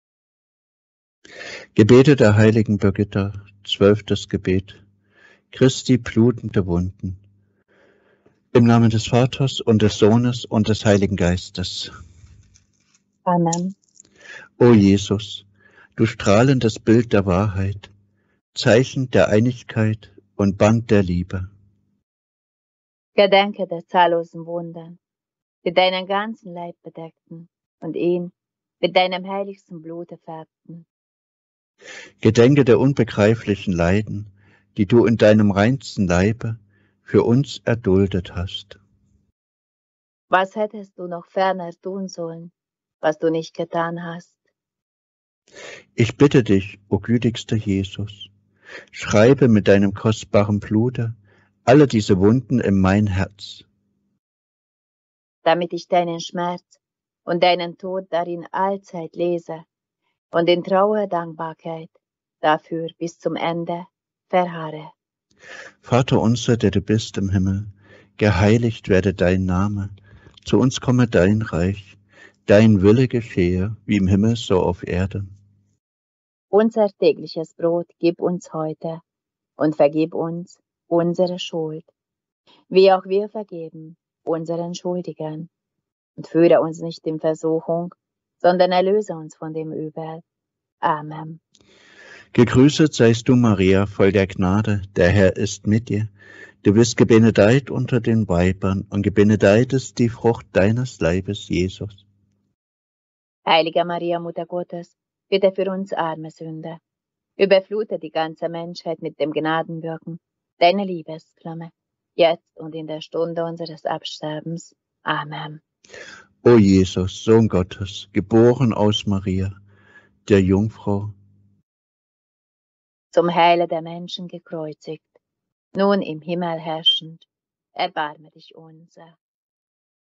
12. Christi blutende Wunden
12. Christi blutende Wunden

O Jesus, du strahlendes Bild der Wahrheit, Zeichen der Einigkeit und Band der Liebe, gedenke der zahllosen Wunden, die deinen ganzen Leib bedeckten und ihn mit deinem heiligsten Blute färbten; gedenke der unbegreiflichen Leiden, die du in deinem reinsten Leibe für uns erduldet hast. Was hättest du noch ferner tun sollen, was du nicht getan hast? Ich bitte dich, o gütigster Jesus, schreibe mit deinem kostbaren Blute alle diese Wunden in mein Herz, damit ich deinen Schmerz und deinen Tod darin allzeit lese und in treuer Dankbarkeit dafür bis zum Ende verharre.

Vater unser, der Du bist im Himmel, geheiligt werde Dein Name, Dein Reich komme, Dein Wille geschehe, wie im Himmel so auf Erden. Unser tägliches Brot gib

uns heute und vergib uns unsere Schuld, wie auch wir vergeben unseren Schuldigern und führe uns nicht in Versuchung, sondern erlöse uns von dem Übel. AMEN.

Gegrüsset seist Du Maria, voll der Gnade, der Herr ist mit Dir! Du bist gebenedeit unter den Weibern und gebenedeit ist die Frucht Deines Leibes Jesus, Heilige Maria, Muttergottes, bitte für uns arme Sünder, überflute die ganze Menschheit mit dem Gnadenwirken Deiner Liebesflamme, jetzt und in der Stunde unseres Todes. Amen.

O Jesus, Sohn Gottes, geboren aus Maria der Jungfrau, zum Heile der Menschen gekreuzigt, nun im Himmel herrschend, erbarme Dich unser!